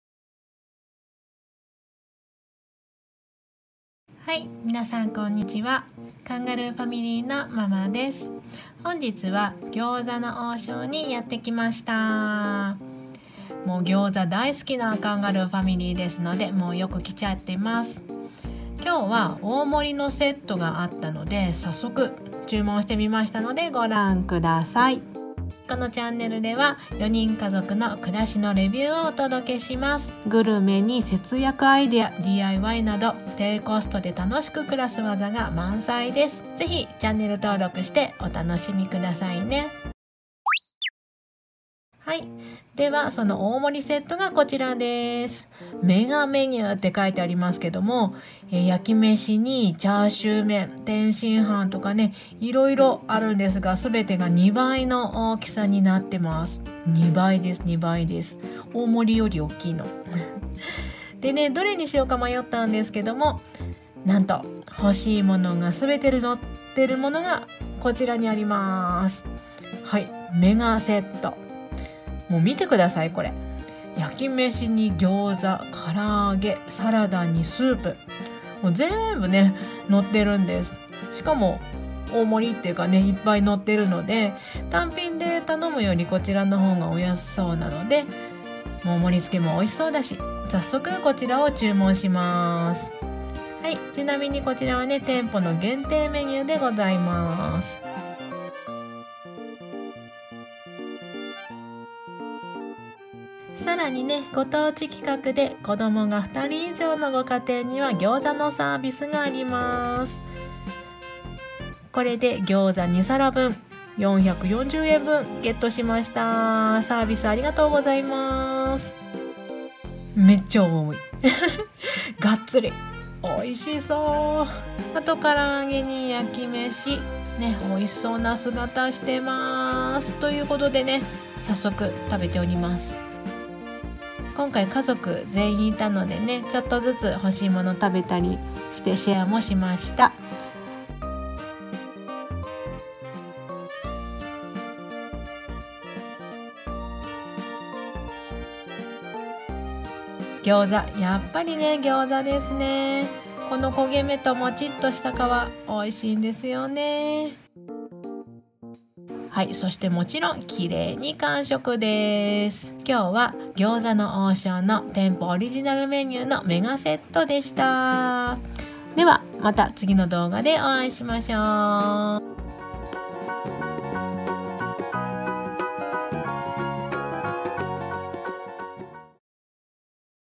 【餃子の王将】MEGAメニューはすべてが2倍でめっちゃお得！（地域限定メニュー）GYOZA OHSHO Super-size meal
Hello かんがるーです♪
家族みんなが大好きな、餃子の王将へ行ってきました！
地域限定メニューのMEGAセットを注文。
大盛りよりも量が多い、2倍盛りがお得でめっちゃ美味しかった♪
See you.

▶︎関連動画
【餃子の王将】無性に食べたくなるギョウザを家族4人で美味しく頂きました！【ワーママ】GYOZA OHSHO Popular Chinese restaurant in Japan.